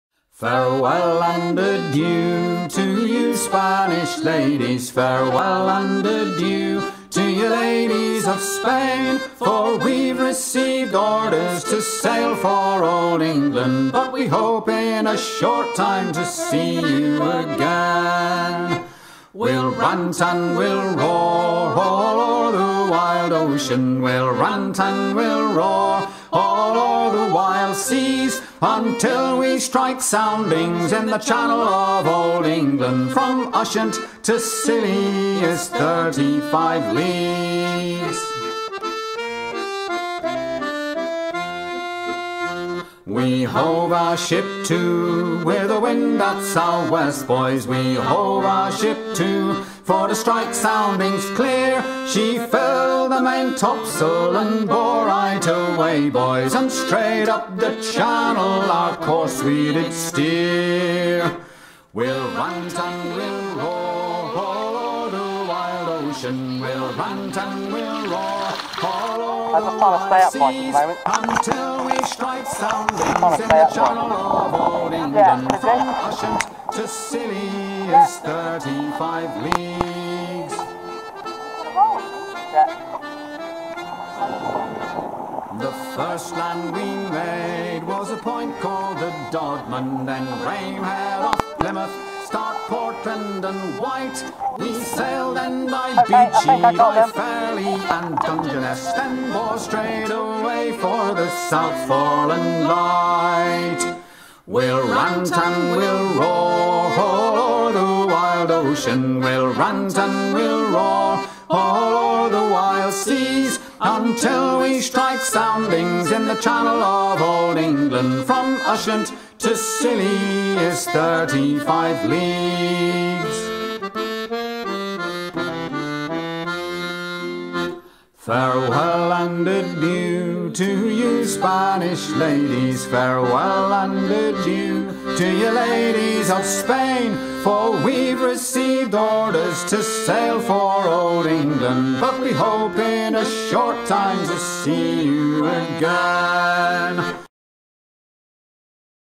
A Visit to the Scilly Bishop by Sea Kayak 2016
A group of six sea kayakers paddle out to the most western point of England, The Bishop Rock Lighthouse in the Scilly Isles off Cornwall. September 2016